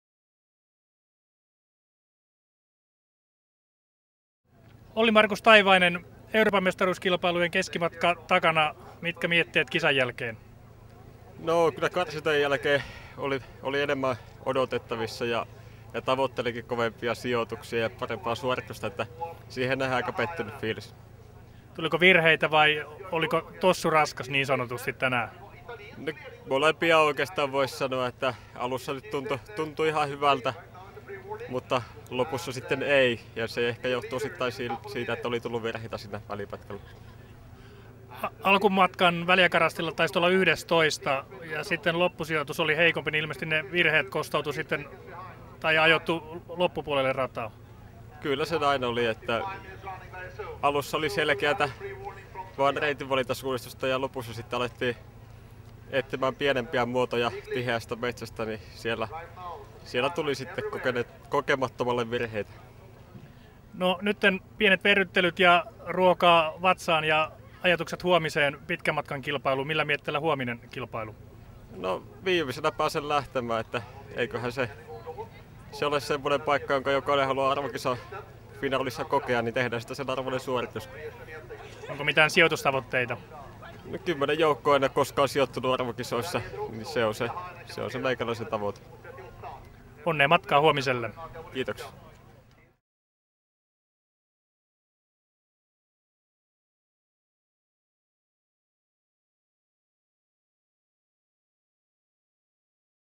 EM-kisat 2012 - Olli-Markus Taivainen
Suunnistaja Olli-Markus Taivaisen haastattelu keskimatkan finaalin jälkeen Ruotsin EM-kisoissa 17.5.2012.